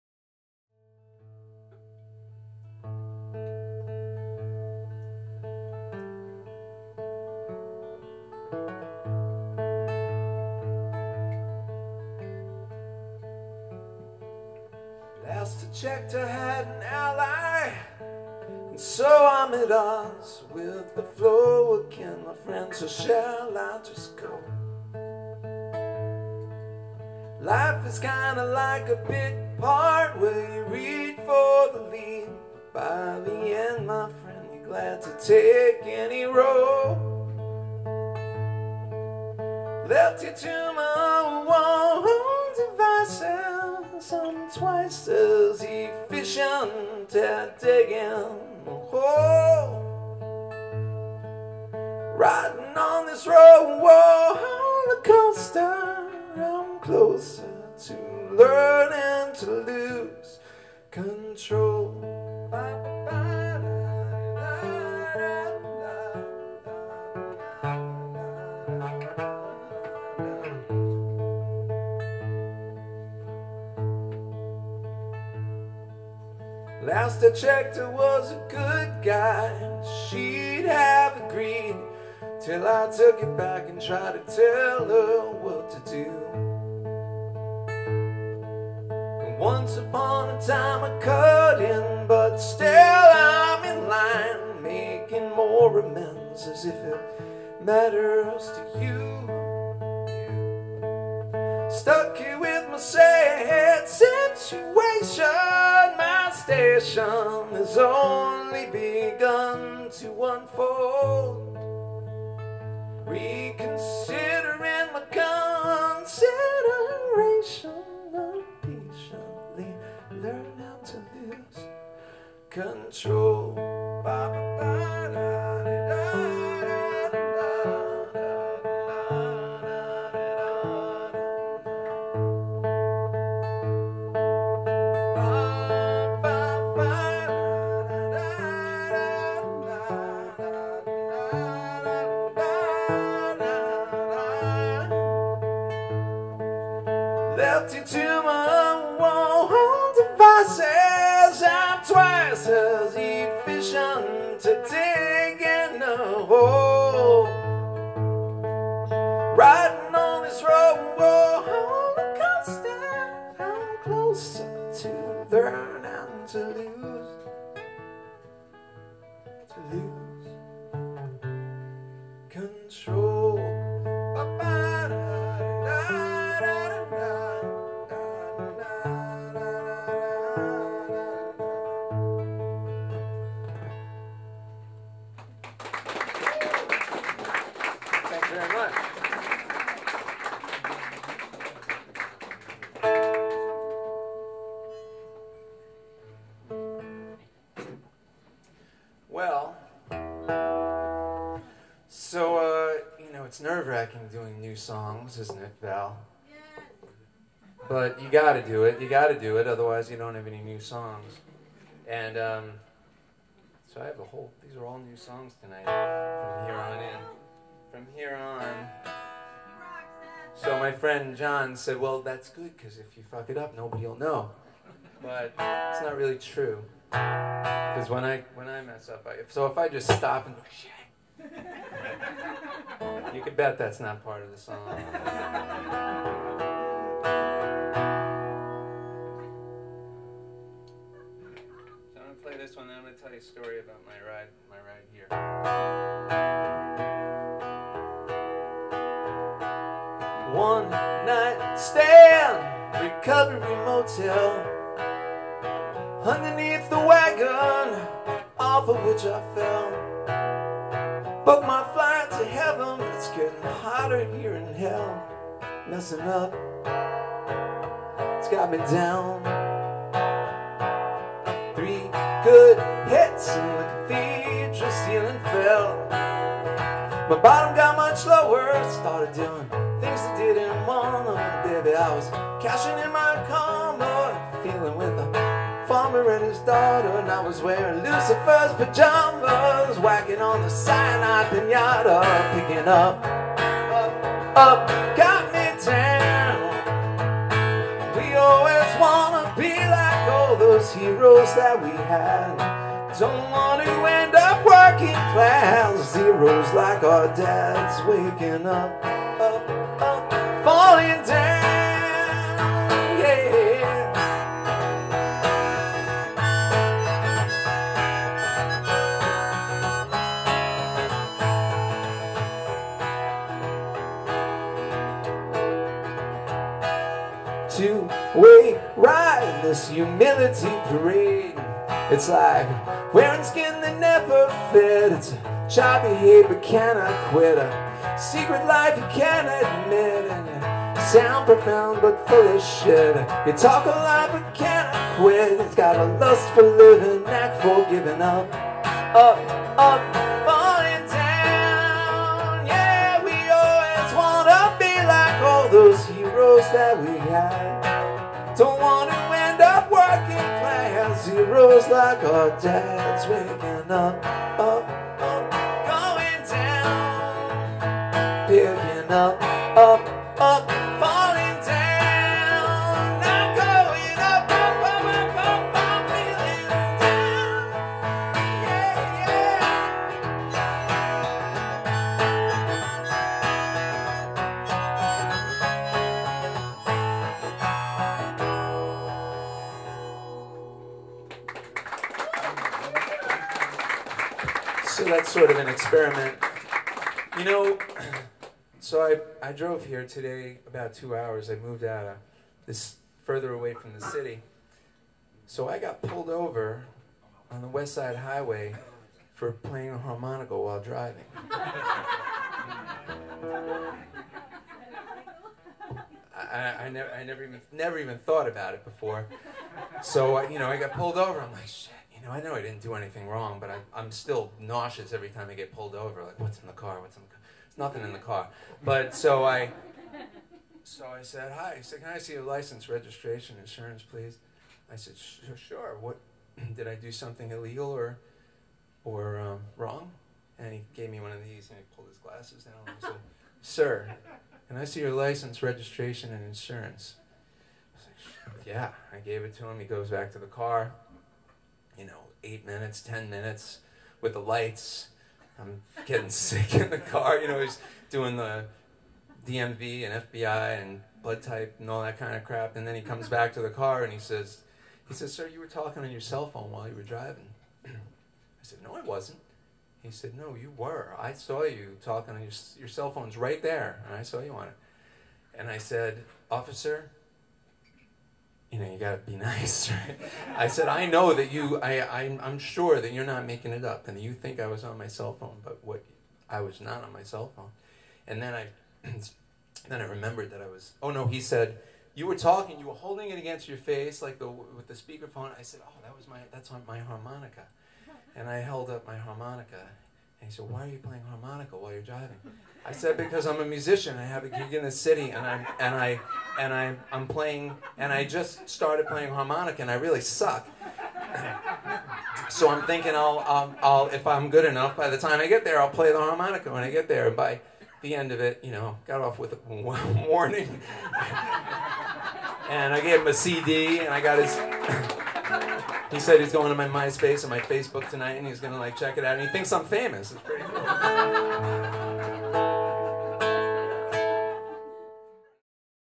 Singer-songwriter Seth Davis @ Cornelia Street Cafe 4/15/09 (HD) Part 1
brand new song written by Seth Davis